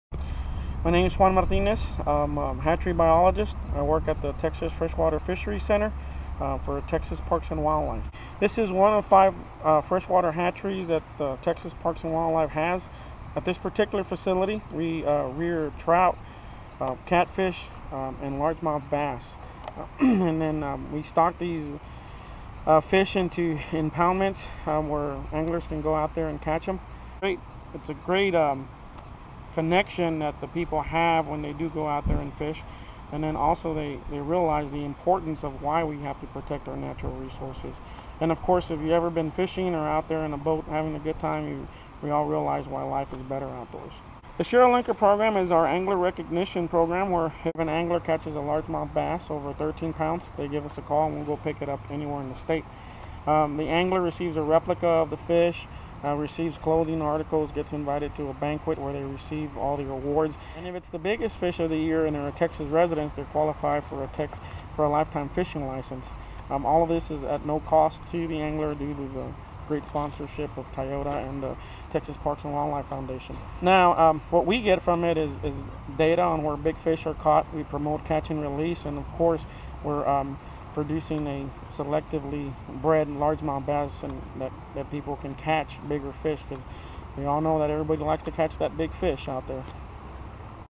Texas Freshwater Fisheries Center
The Texas Freshwater Fisheries Center in Athens, Texas houses one of five state fish hatcheries breeding Trout, Catfish, and Largemouth Bass with Largemouth Bass being their primary focus producing 3-4 million fingerlings a year!